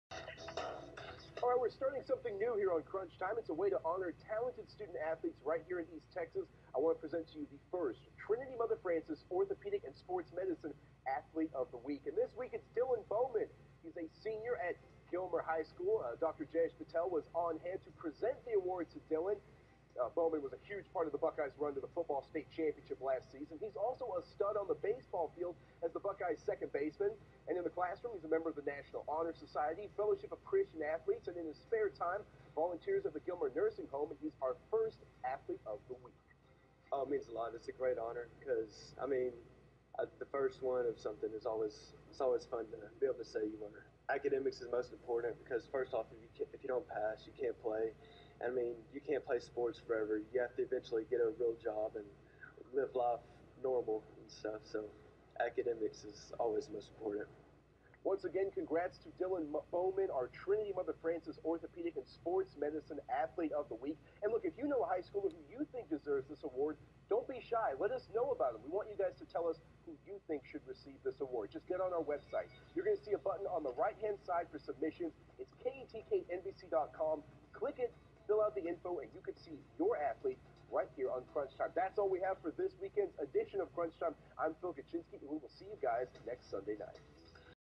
Athlete of the Week - Dylan Bowman
(KETK NBC 56 - Tyler, TX)

This weeks Trinity Mother Frances Orthopedics and Sports Medicine Athlete of the Week was presented by Dr. Jayesh Patel to Dylan Bowman of the Gilmer Buckeyes.